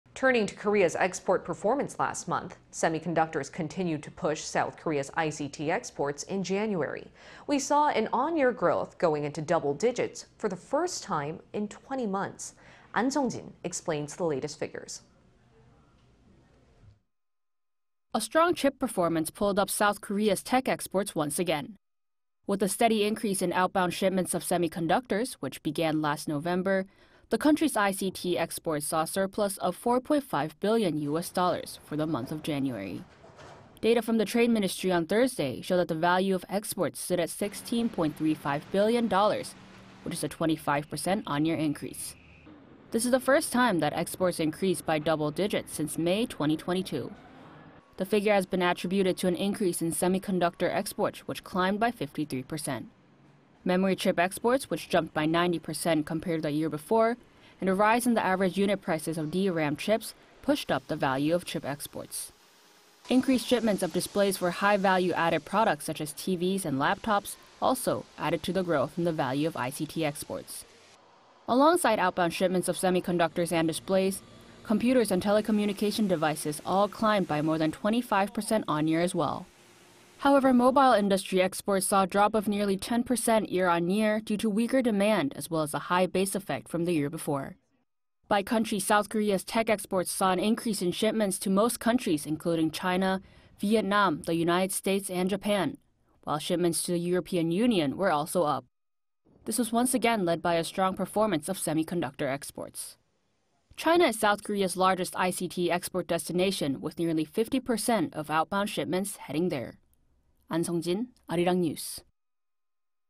S. Korea's tech exports see double digit y/y increase for the first time in 20 months
1월 ICT 수출 전년 比 25.1%↑ 반도체 성장으로 20개월 만에 두자릿수 상승

Turning to Korea's export performance last month.
Semiconductors continued to push South Korea's ICT exports in January.
We saw an on-year growth going into double digits for the first time in 20 months.
Ahn Sung-jin explains the latest figures.

A strong chip performance pulled up South Korea's tech exports once again.
With a steady increase in outbound shipments of semiconductors which began last November, the country's ICT exports saw a surplus of 4-point-5 billion U.S. dollars for the month of January.
Data from the Trade Ministry on Thursday showed that the value of exports stood at 16-point-3-5 billion dollars, which is a 25 percent on-year increase.
This is the first time that exports increased by double digits since May 2022.
The figure has been attributed to an increase in semiconductor exports which climbed by 53 percent.
Memory chip exports, which jumped by 90 percent compared to the year before, and a rise in the average unit prices of DRAM chips pushed up the value of chip exports.
Increased shipments of displays for high-value-added products such as TVs and laptops also added to the growth in the value of ICT exports.
Alongside outbound shipments of semiconductors and displays, computers and telecommunication devices all climbed by more than 25 percent on-year as well.
However, mobile industry exports saw a drop of nearly 10 percent year on year due to weaker demand as well as a high-base effect from the year before.
By country, South Korea’s tech exports saw an increase in shipments to most countries including China, Vietnam, the United States, and Japan, while shipments to the European Union were also up.
This was once again led by a strong performance of semiconductor exports.
China is South Korea's largest ICT export destination – with nearly 50 percent of outbound shipments heading there.

Ahn Sung-jin, Arirang News.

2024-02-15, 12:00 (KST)